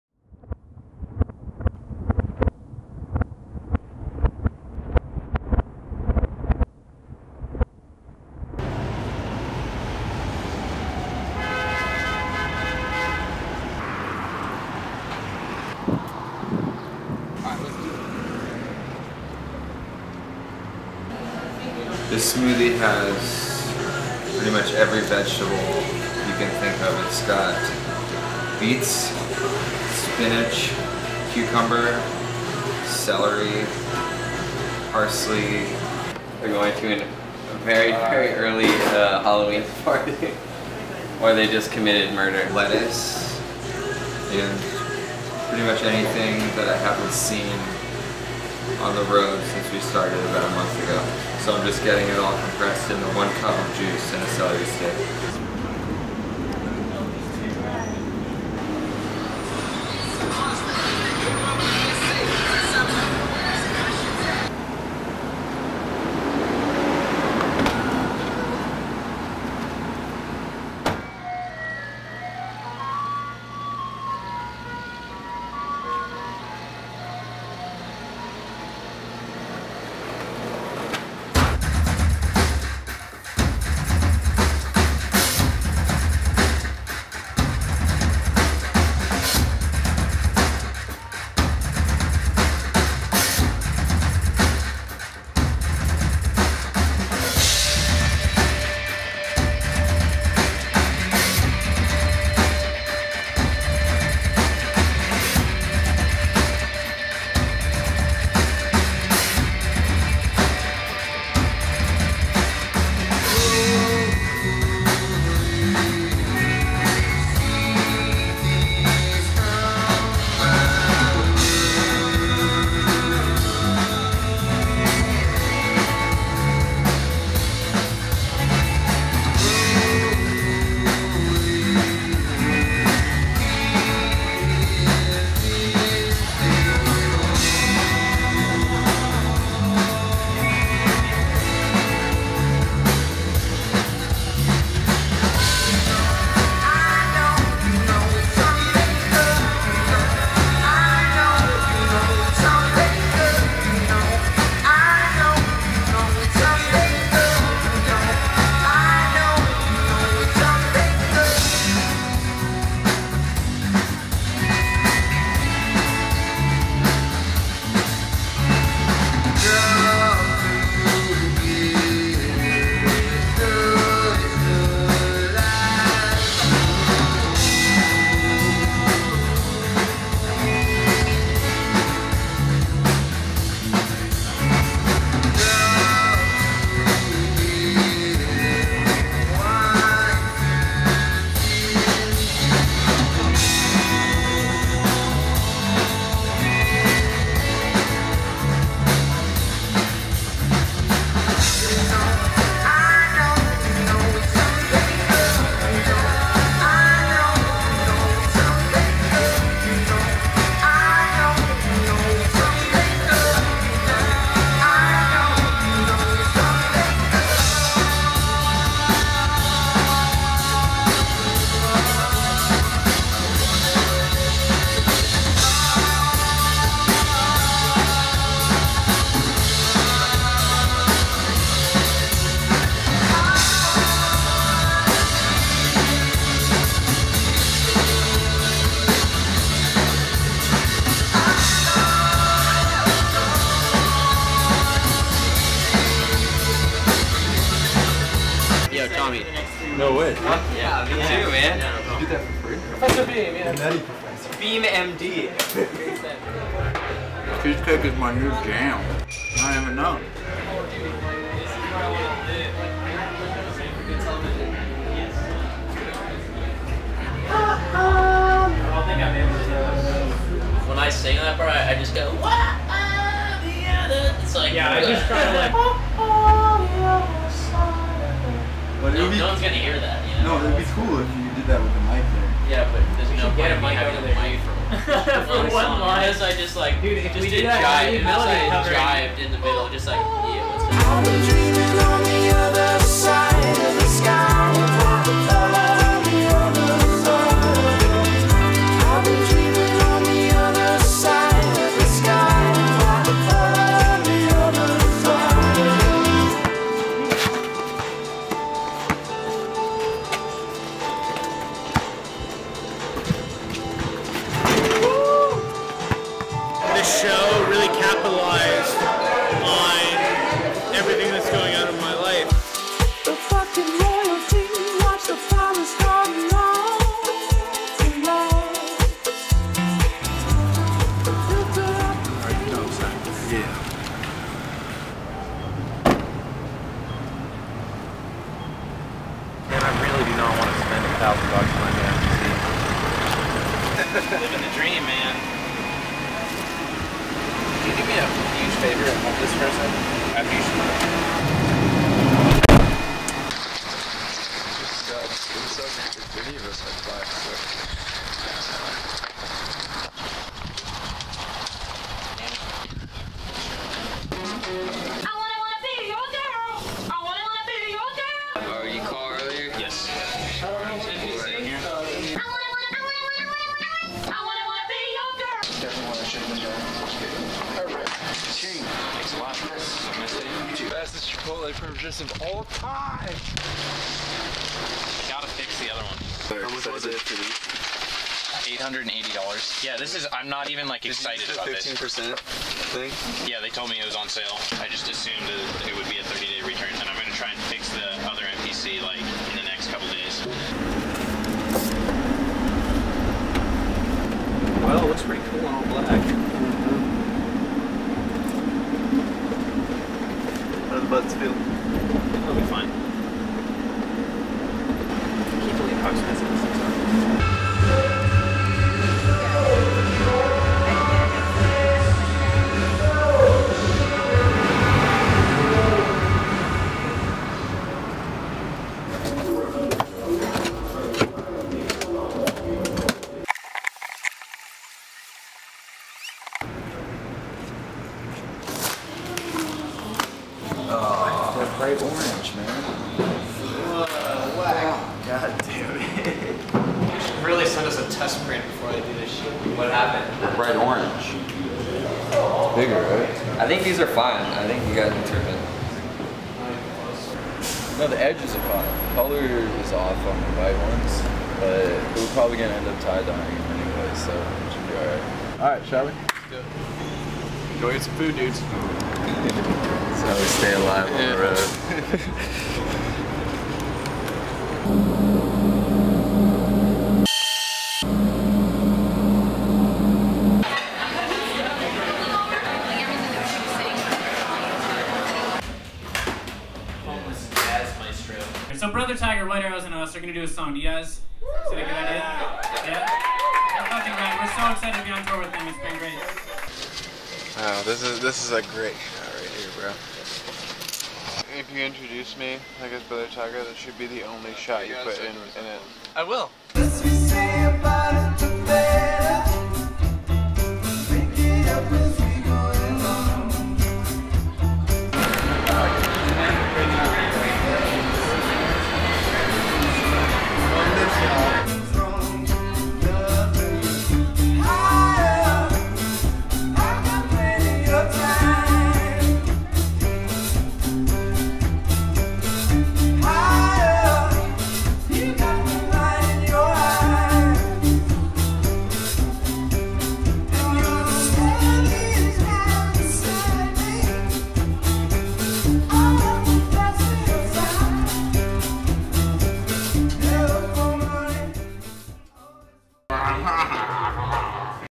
Beat Connection/White Arrows Tour Episode 3
Beat Connection and White Arrows are on the road together. This documentary series of videos details their journey across the United States, and is being produced and released on the road as the tour unfolds.

Directed by Jon Meyer
Assistant Director Rachel Sharkeye
Produced by Votiv